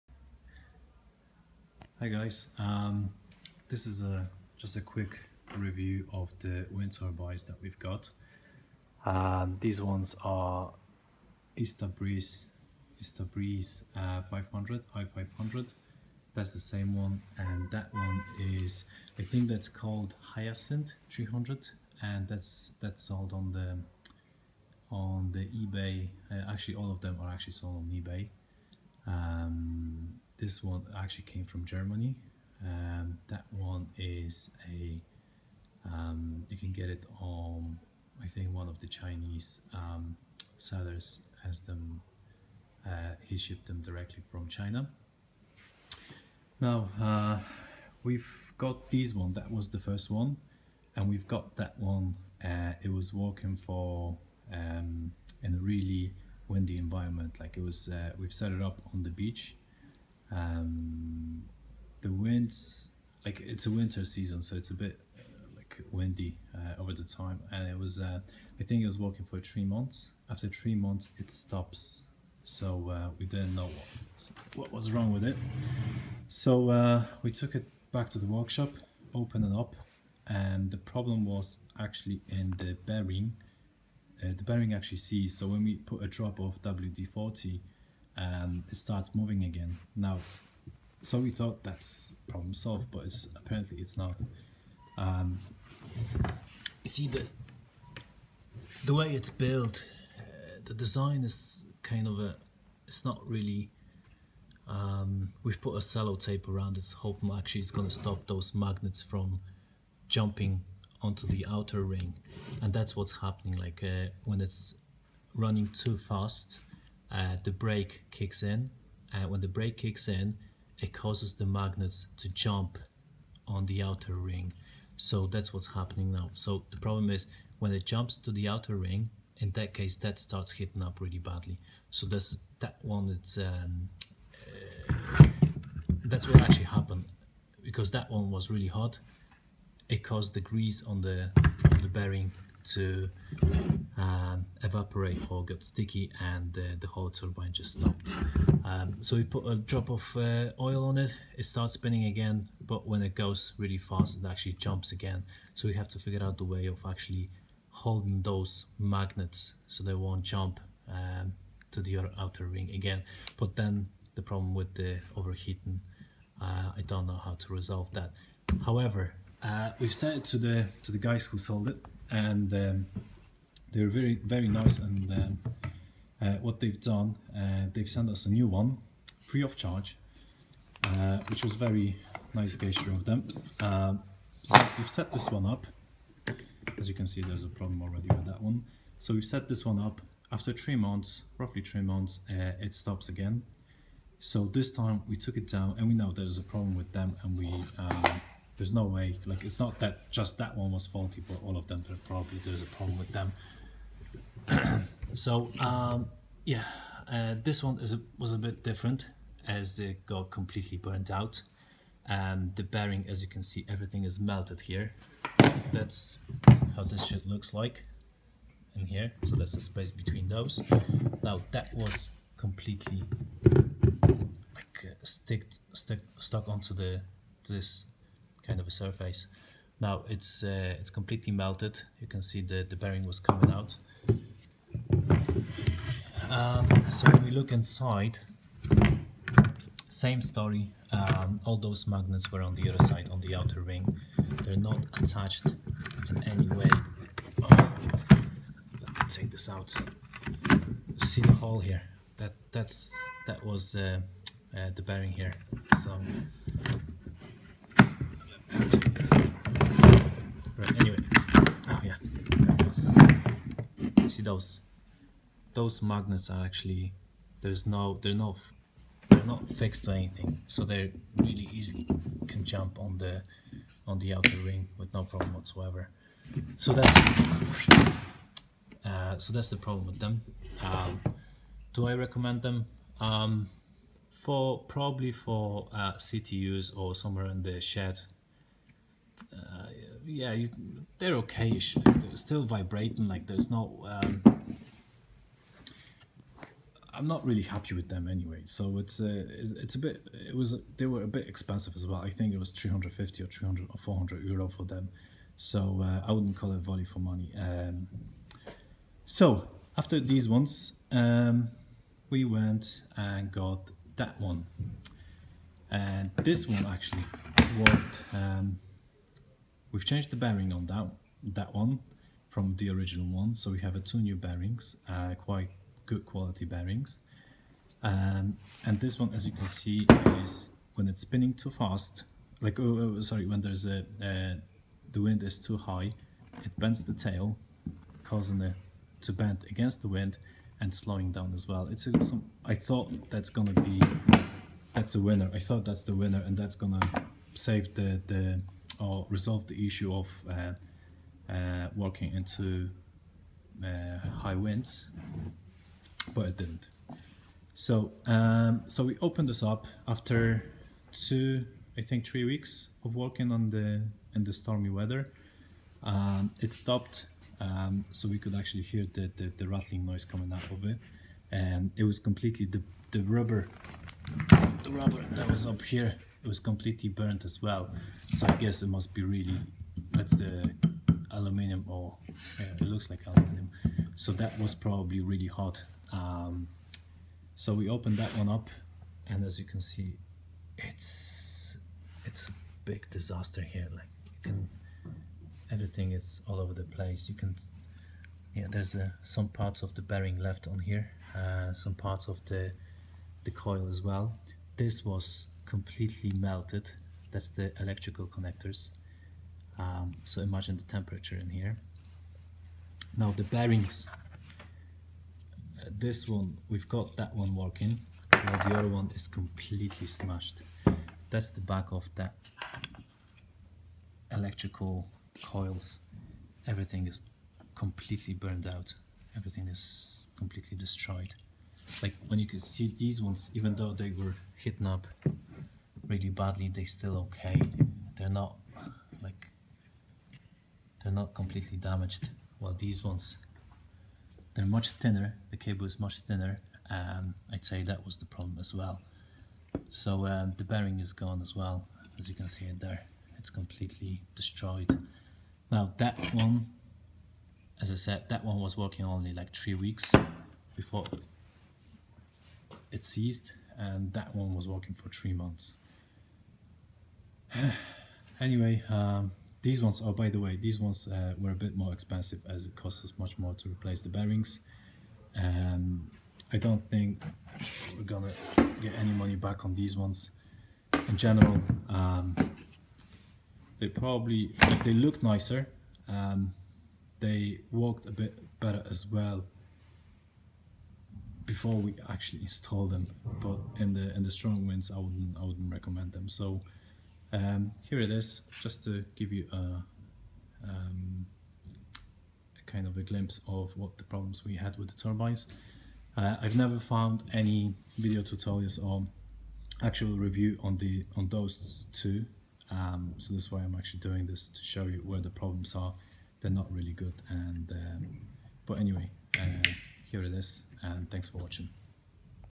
Review of ista breeze i500 and hyacinth 300 wind turbines independent test
This video was done to show what has happened to Ista Breeze i500 Turkish wind turbine after 3 months of use and also Hiacynth 300 - Chinese wind turbine after 3 weeks... We are still looking for one that would survive in windy environments - so if there is a manufacturer / reseller out there confident enough to trial it - feel free to contact me...